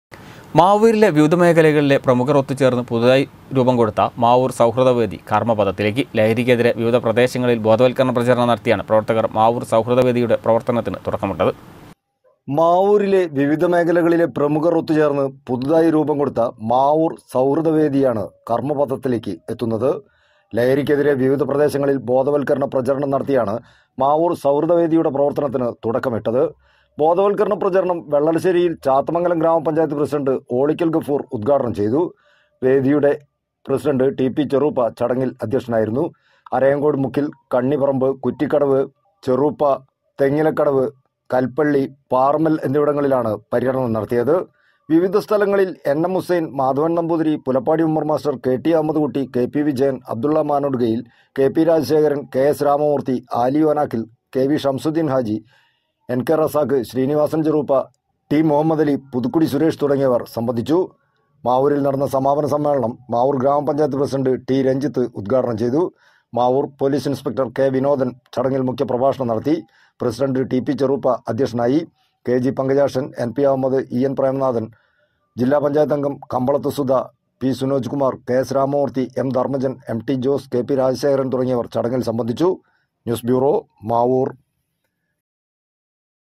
മാവൂരിലെ വിവിധ മേഖലകളിലെ പ്രമുഖർ ഒത്തുചേർന്ന് പുതുതായി രൂപംകൊടുത്ത മാവൂർ സൗഹൃദ വേദി
മാവൂരിലെ വിവിധ മേഖലകളിലെ പ്രമുഖർ ഒത്തുചേർന്ന് പുതുതായി രൂപംകൊടുത്ത മാവൂർ സൗഹൃദ വേദി കർമപഥത്തിലേക്ക്. ലഹരിക്കെതിരെ വിവിധ പ്രദേശങ്ങളിൽ ബോധവത്കരണ പ്രചരണം നടത്തിയാണ് പ്രവർത്തകർ  മാവൂർ സൗഹൃദ വേദിയുടെ പ്രവർത്തനത്തിന് തുടക്കമിട്ടത്.